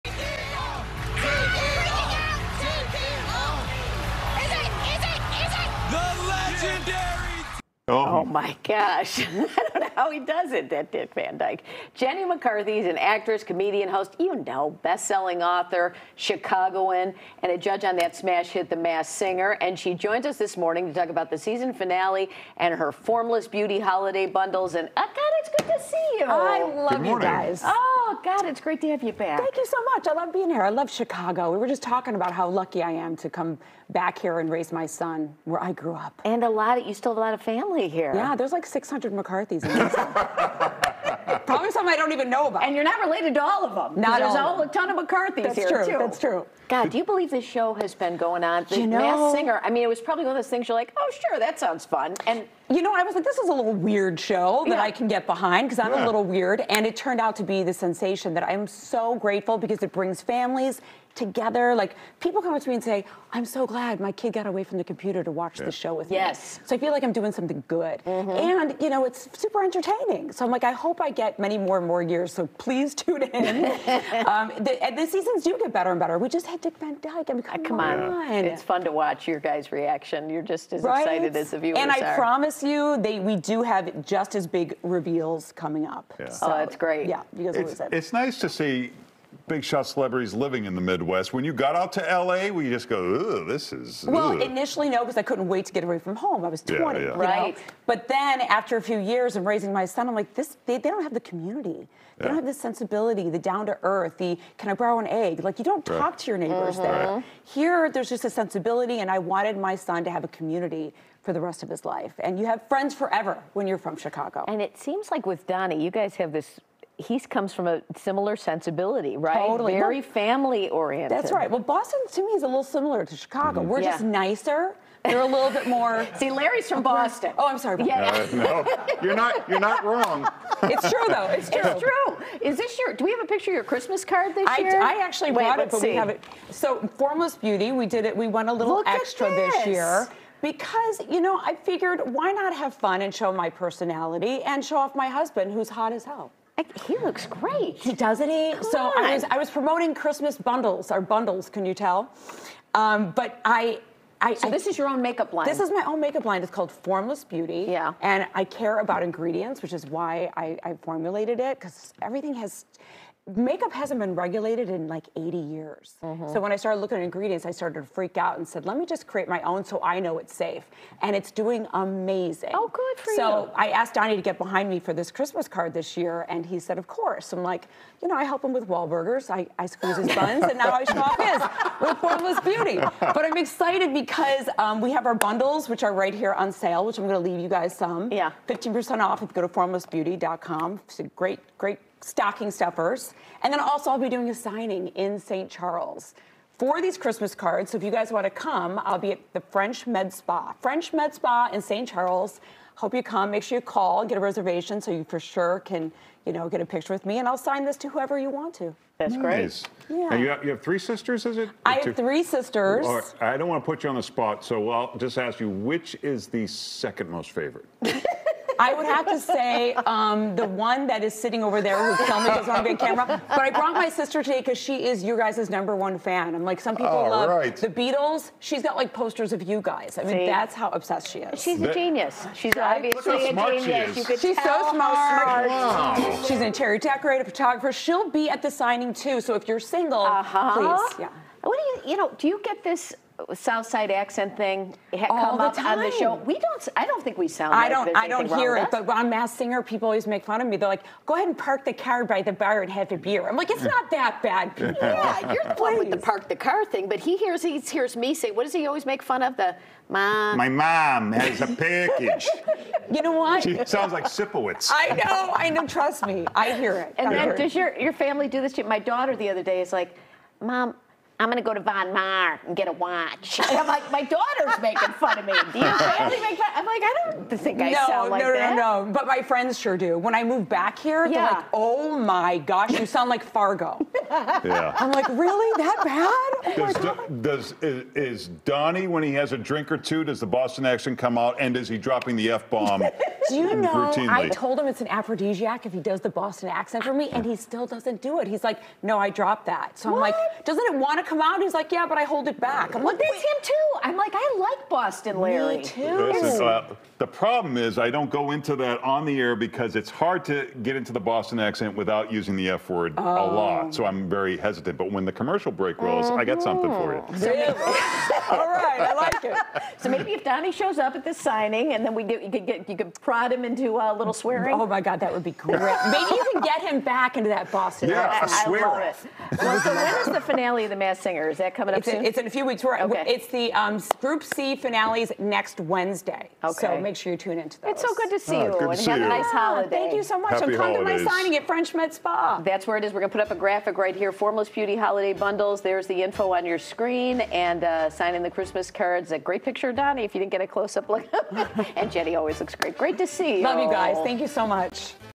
Chicago's very own Jenny McCarthy talks about "The Masked Singer" finale and her Formless Beauty Hol
She rocks!